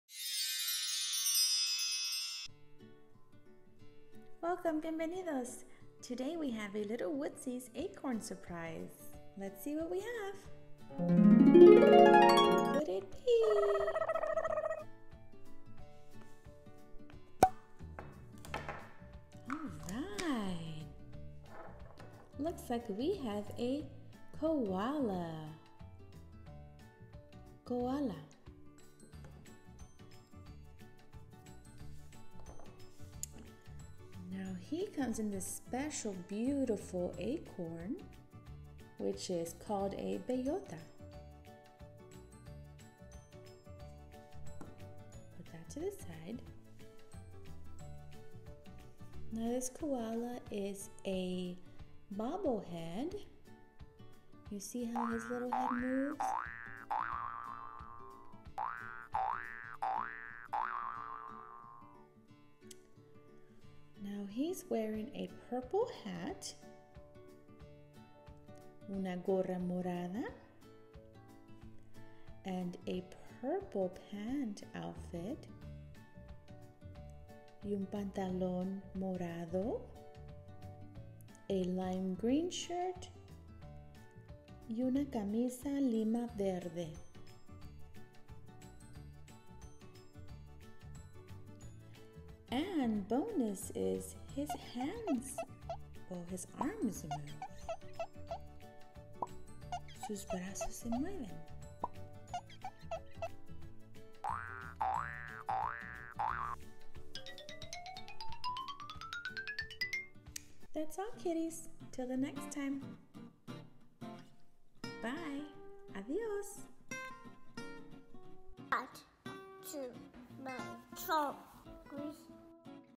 Li'l Woodzeez Acorn Surprise, Blind Bag #2
Li'l Woodzeez Acorn Surprise, Blind Bag #2. This acorn surprise teaches colors and words through our Li'l Woodzeez friend.